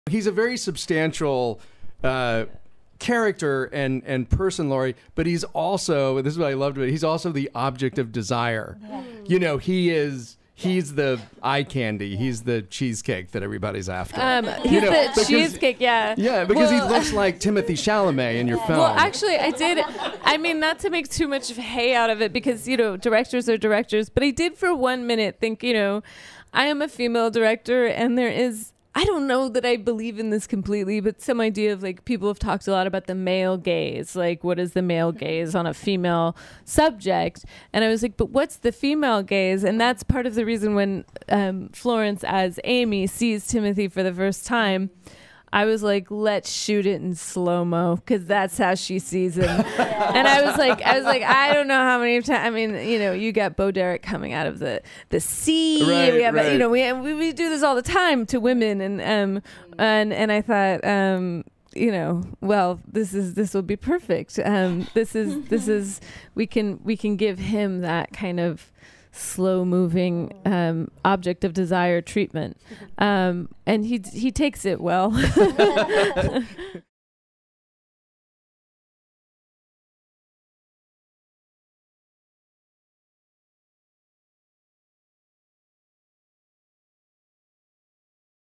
Female Gaze Trumps the Male Gaze in 'Little Women'
For once, it's all about the female gaze in Greta Gerwig's 'Little Women.'